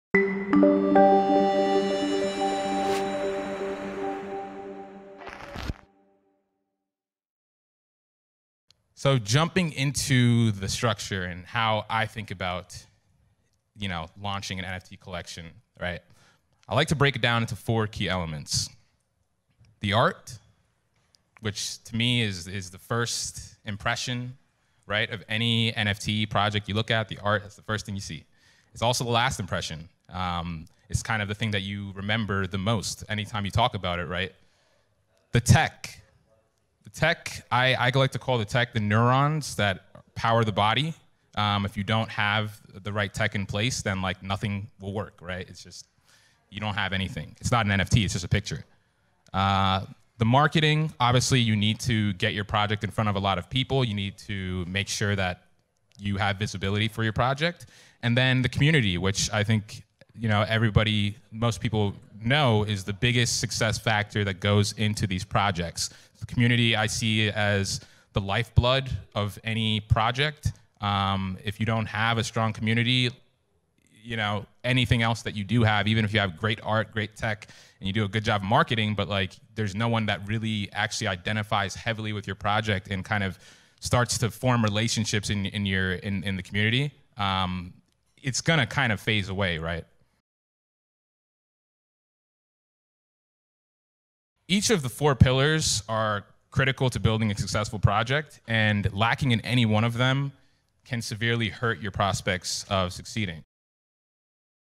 How to Launch an NFT Collection | Key Elements for Success | SXSW 2022
In this video, one of our founders Emmanuel Udotong breaks down the process of launching an NFT collection into 4 key elements:
1.) Art
2.) Tech
3.) Marketing
4.) Community